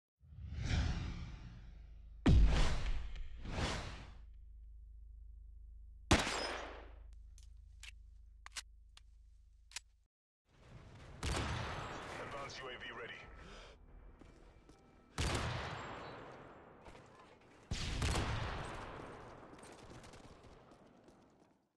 xCodShots video submission 3
Two regular quickscopes followed by what I would describe as a reverse-helicopter shot. Kind of ironic how I shoot a guy going up to a relatively well-known trickshot spot from a spot that barely anyone trickshots on :)